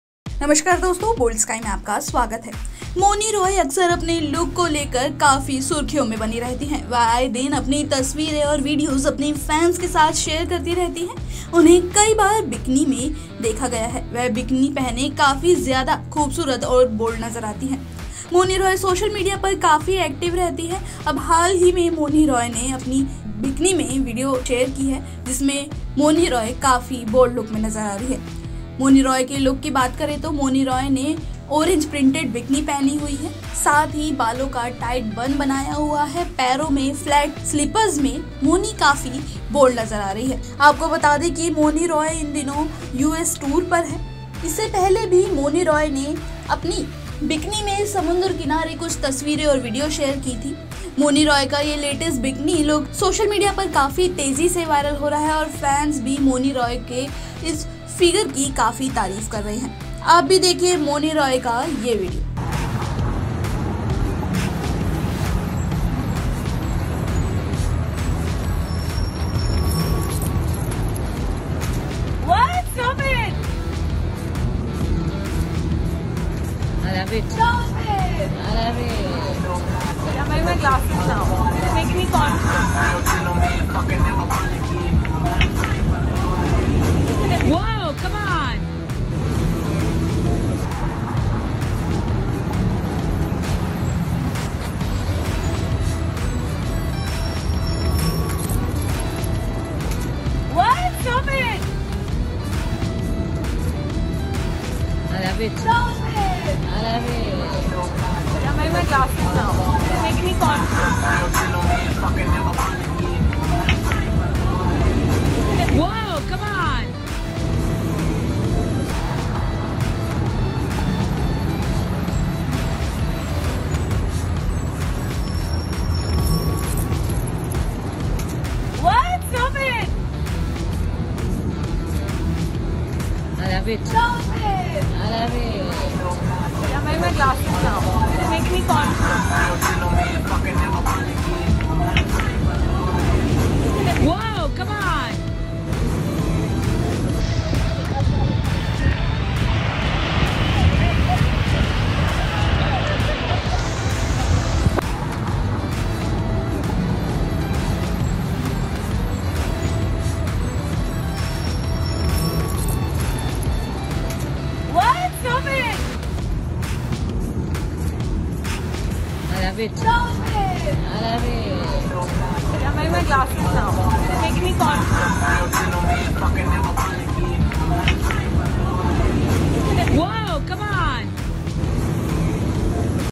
Mouni Roy Orange Bikini में Miami Road पर Dance करते Video Viral । Boldsky
Mouni Roy Orange Bikini में Miami Road पर Dance करते Video Viral: मौनी रॉय इन दिनों यूस टूर पर है. मौनी रॉय सोशल मीडिया पर काफी एक्टिव रहती है. मौनी ने हाल ही में एक वीडियो शेयर किया जिसमे मौनी ऑरेंज बिकिनी में सड़क पर डांस करती हुई नज़र आ रही है. आप भी देखिये मौनी का ये वीडियो:-

Video Viral of Mouni Roy dancing on Miami Road in Orange Bikini: Mouni Roy is on a US tour these days. Mouni Roy remains very active on social media. Mouni recently shared a video in which Mouni is seen dancing on the road in orange bikini. You also see this video of Mouni